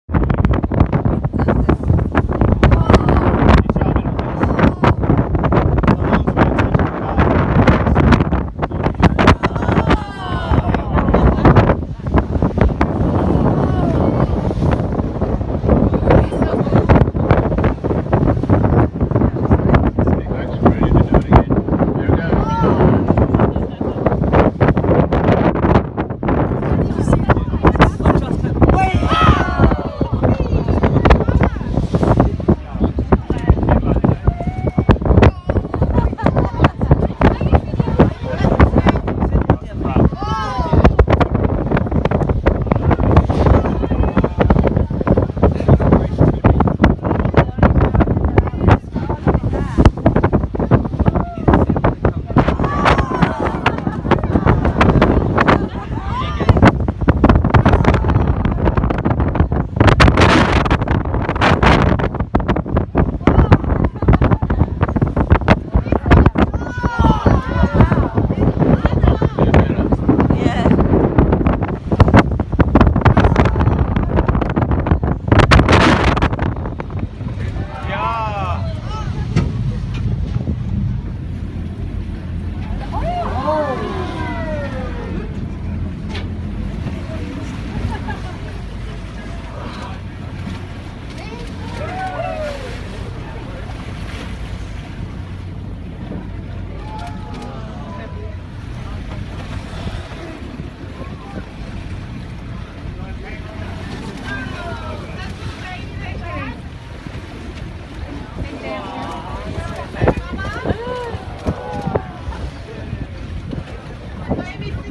Hervey Bay - Whale watching
On the 29th September 2016 we arrived in Hervey Bay after spending some time in Agnes Water and the Town of 1770. On the route down we decided to go whale watching - what a great morning it turned out to be! The weather was pretty rough, which was a bit unnerving but the guide reassured us that this would encourage the whales and their calves to be more active than usual, in pretty sure that this was the case! Check it out!